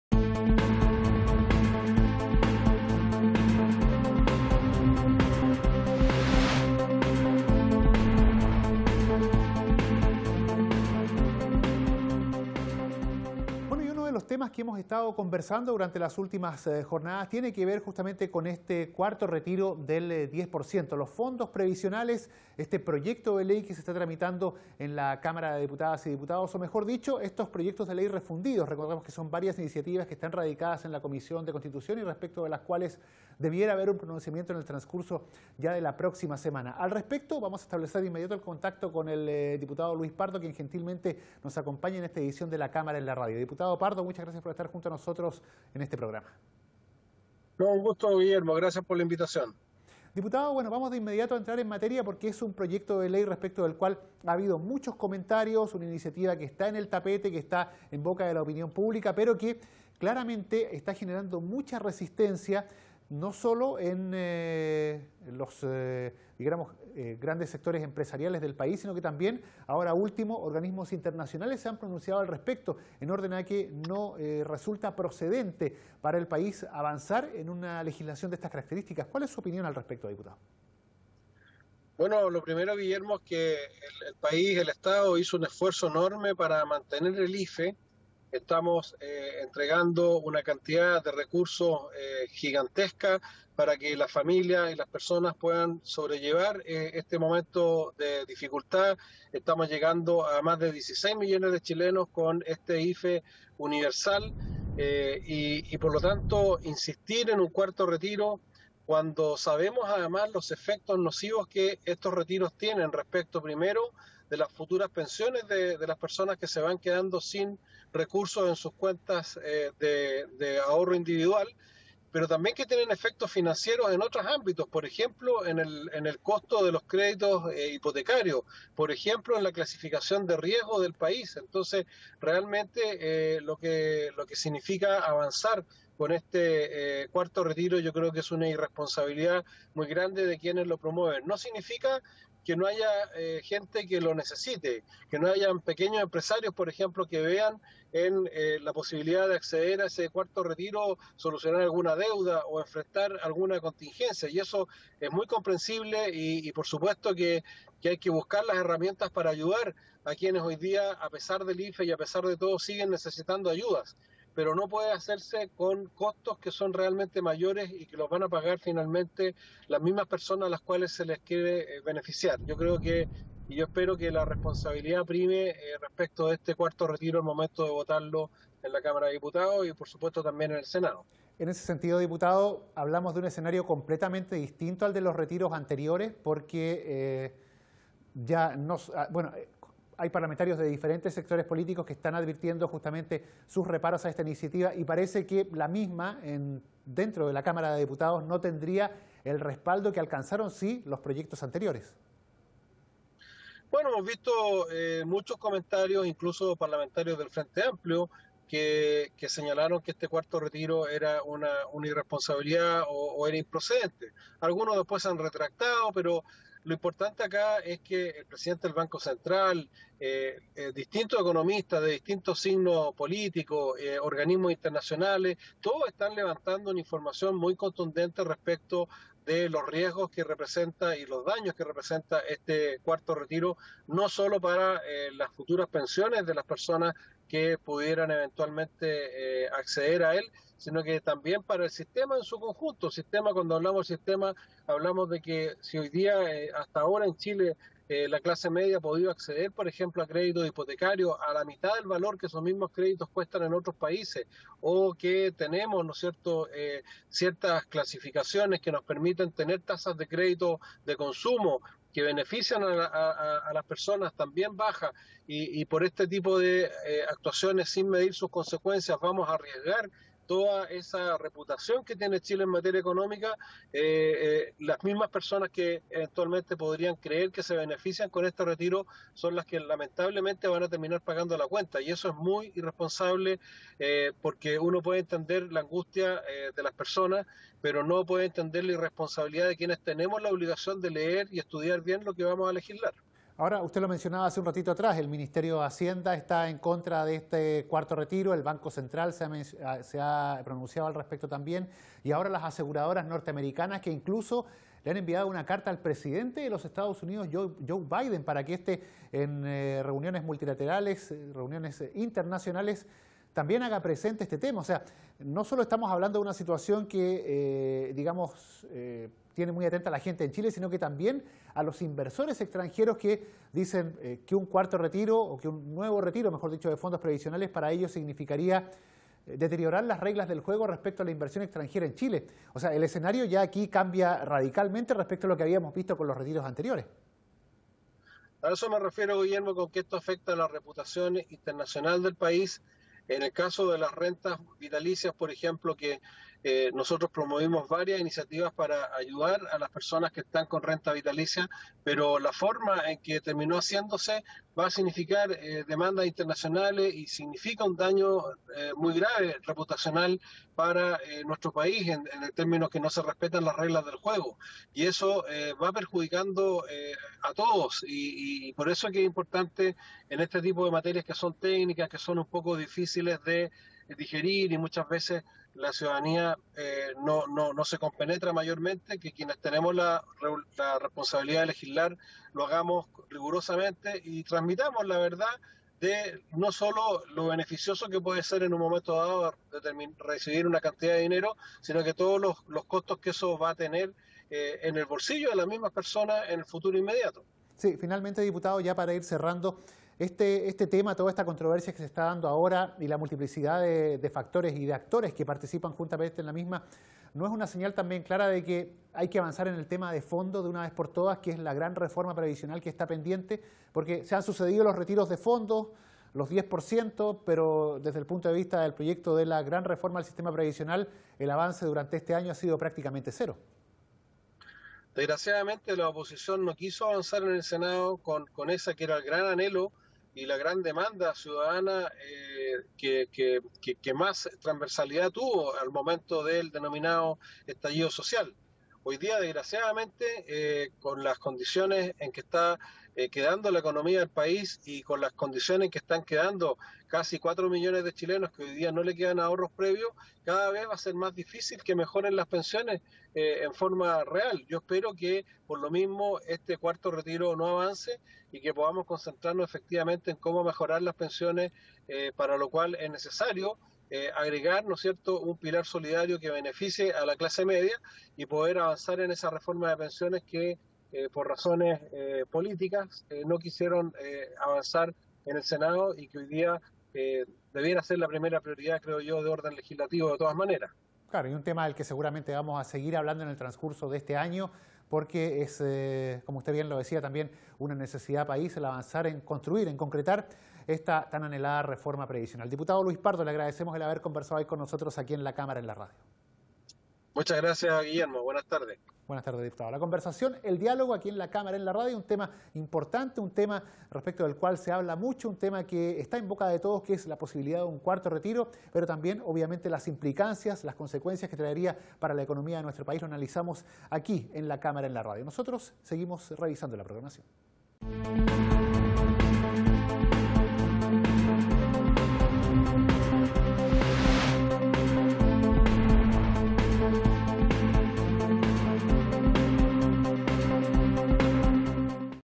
Diputado Pardo: “Esperemos que la Cámara rechace el cuarto retiro, por el perjuicio que traería”
El parlamentario (RN) sostuvo que un nuevo retiro de fondos previsionales no solo deteriorará más el sistema de pensiones, sino que además provocará un grave daño desde el punto de vista de la imagen internacional de Chile, desde el punto de vista de la inversión extranjera.